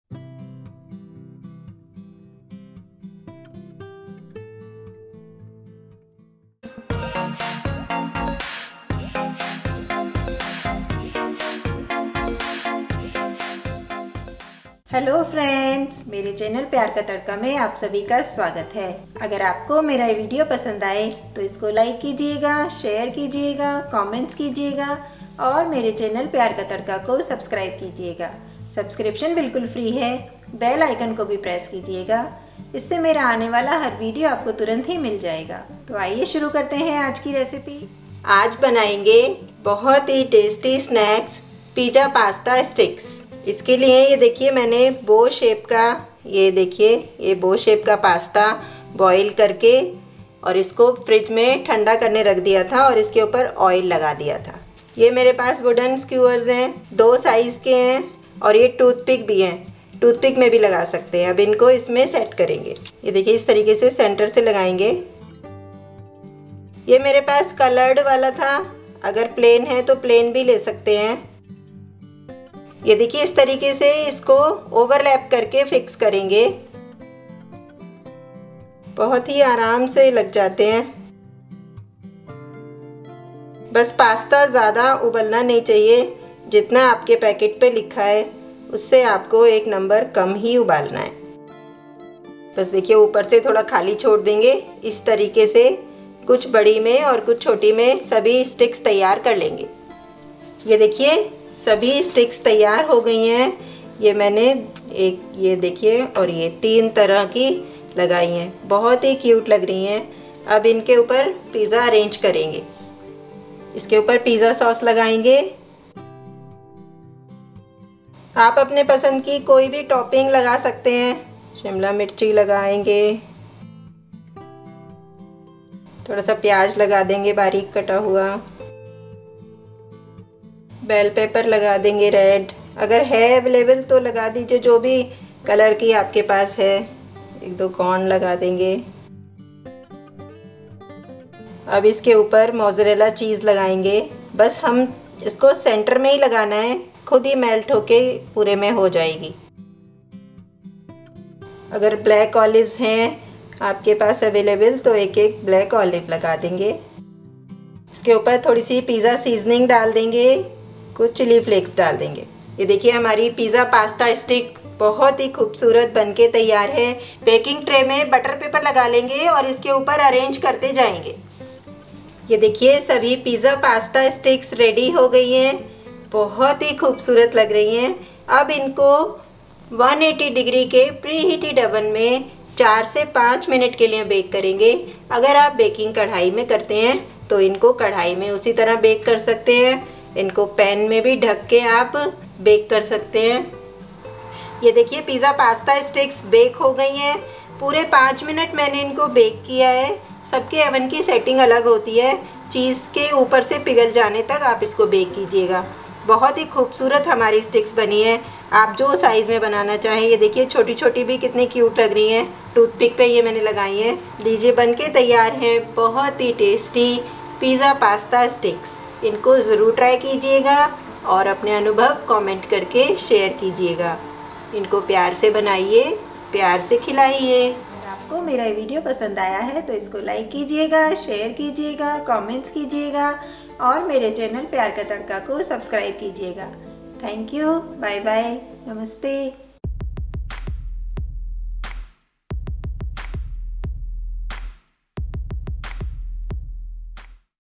Pizza Pasta Sticks | Unique 5 Min Snacks Recipe for Kids
Hello Friends,

In this video I teach you how to make Pizza Pasta Sticks.